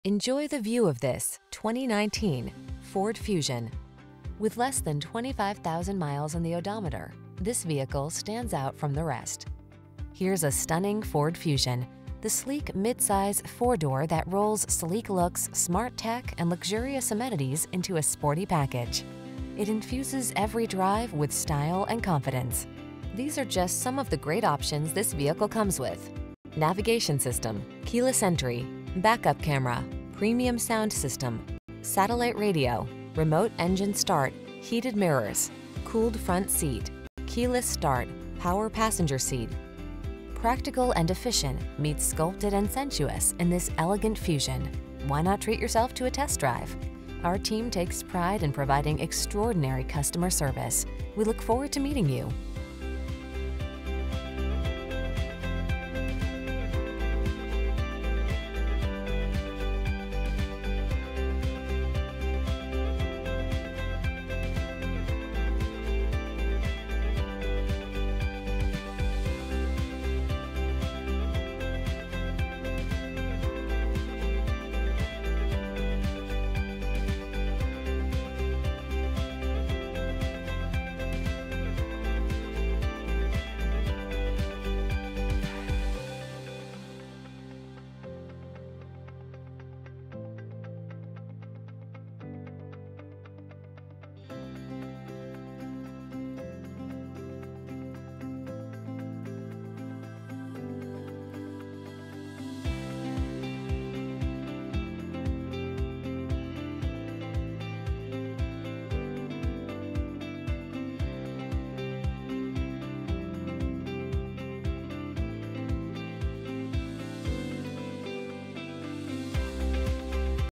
2019 Ford Fusion Energi Carson City, Reno, Lake Tahoe, Northern Nevada, Susanville, NV PH3844A
Used 2019 Ford Fusion Energi available in Carson City, Nevada at Michael Hohl Subaru. Servicing the Reno, Lake Tahoe, Northern Nevada, Susanville, NV area.
2019 Ford Fusion Energi Titanium - Stock#: PH3844A - VIN#: 3FA6P0SU1KR184993

2019 Ford Fusion Energi Titanium**CARFAX CLEAN**, I4 Hybrid.eCVT FWD Odometer is 6011 miles below market average! I4 HybridThis vehicle comes with a 90 day or 3,000 mile limiited powertrain warranty!! This vehicle is NOT sold as-is!!!